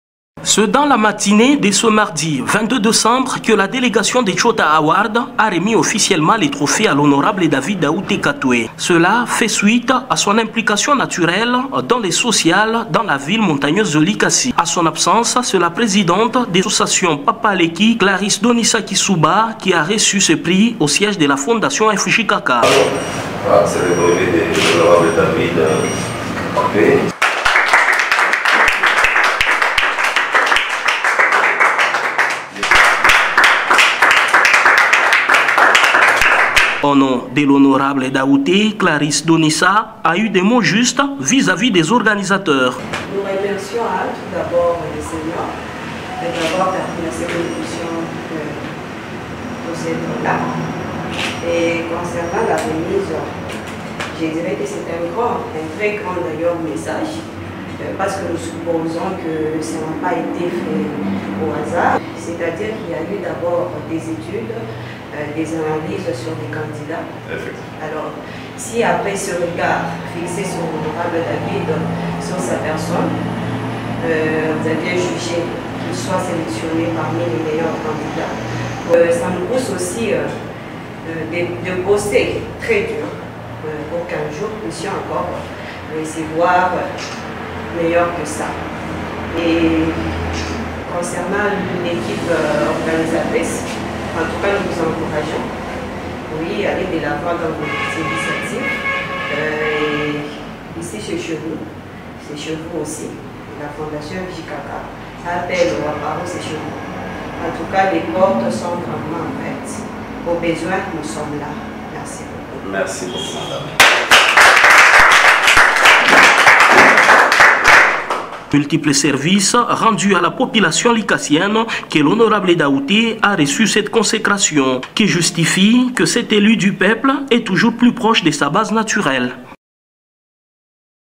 SOCIAL : REMISE DU TROPHEE A DAVID DAUTE
Remise de brevet de mérite à l’honorable David DAUTE, celui qu’on appelle affectueusement PAPA LEKI, le mardi 22 décembre par le TSHOTA AWARD.